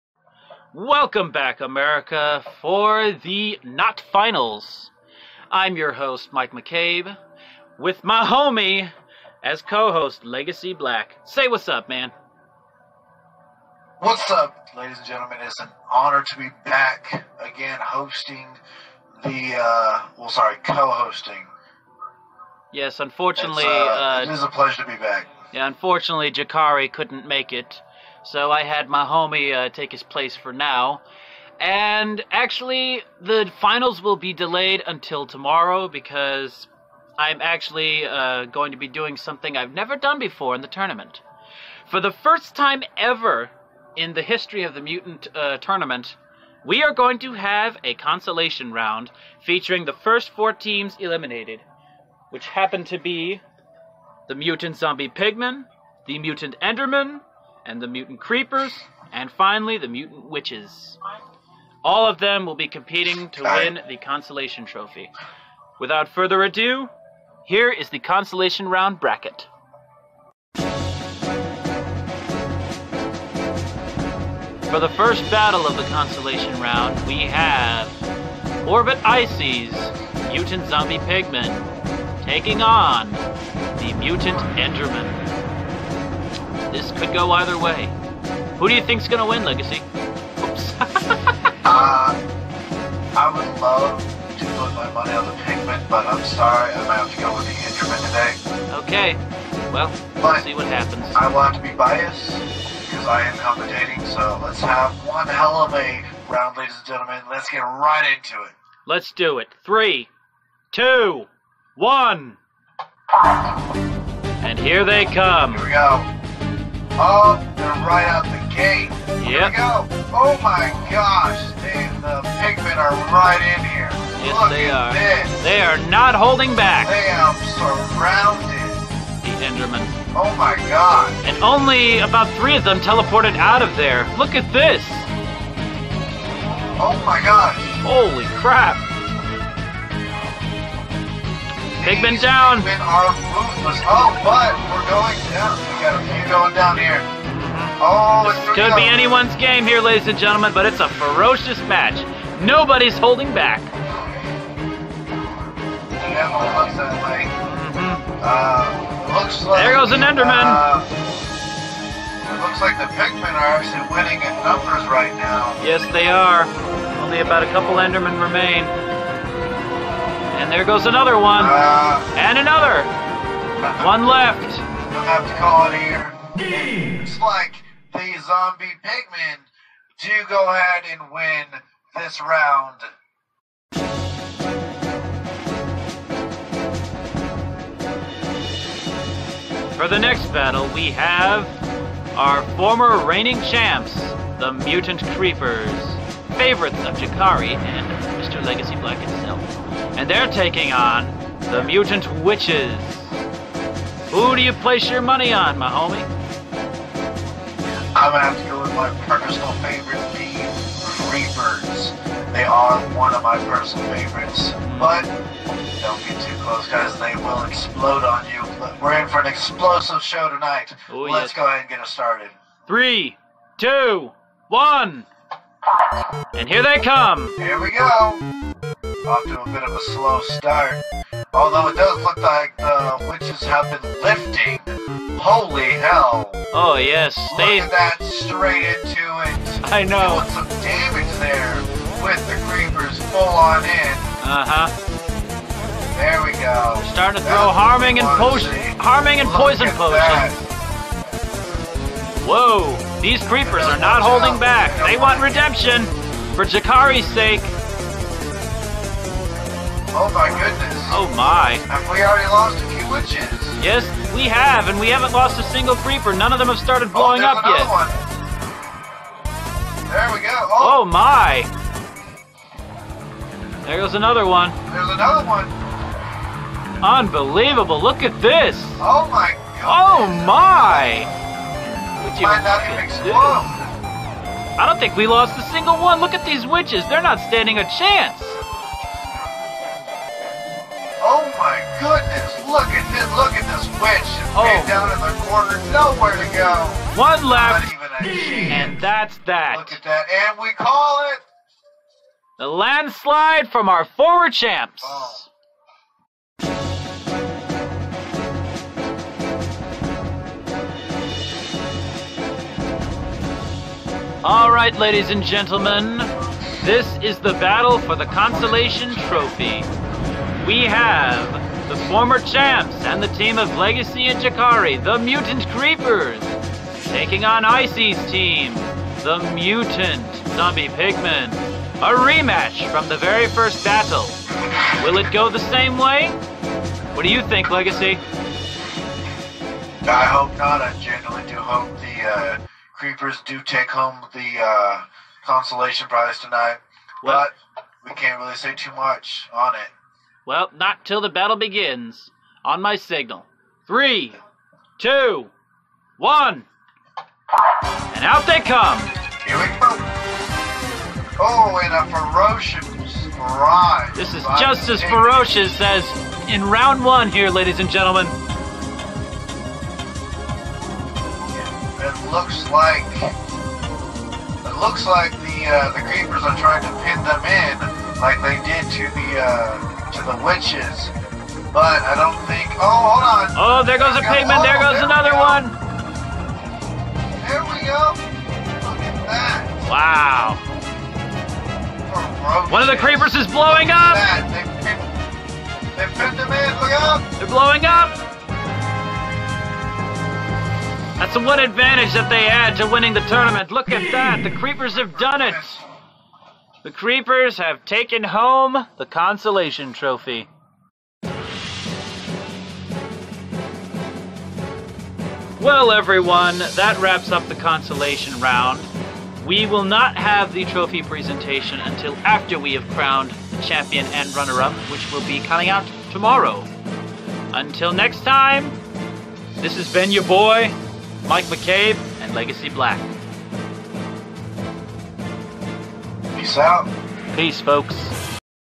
Mutant Monster Battle Tournament Season 2 Consolation Round
The first four teams that were eliminated at the beginning of the tournament have an opportunity to come back and claim the consolation round trophy.